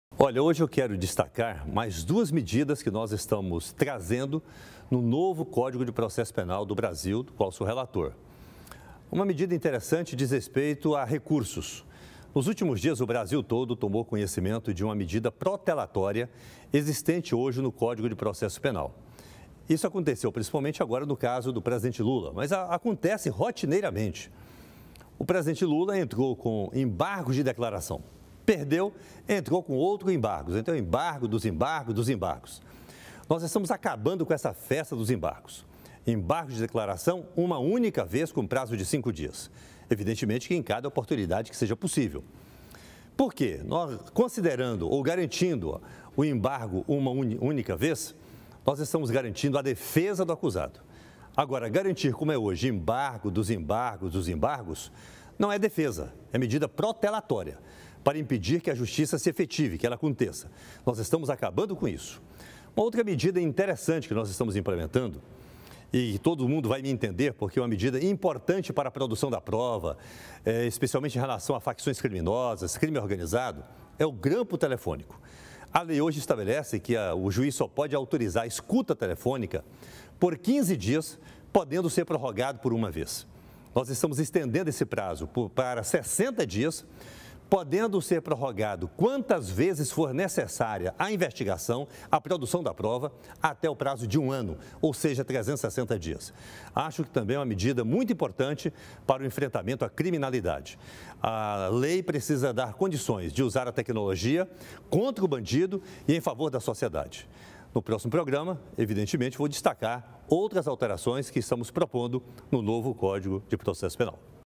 BG - Novo código de processo civil brasileiro - 28-04-2018
E agora você vai conhecer mais sobre duas medidas do novo código do processo civil brasileiro. Quem explica pra gente é deputado federal João Campos.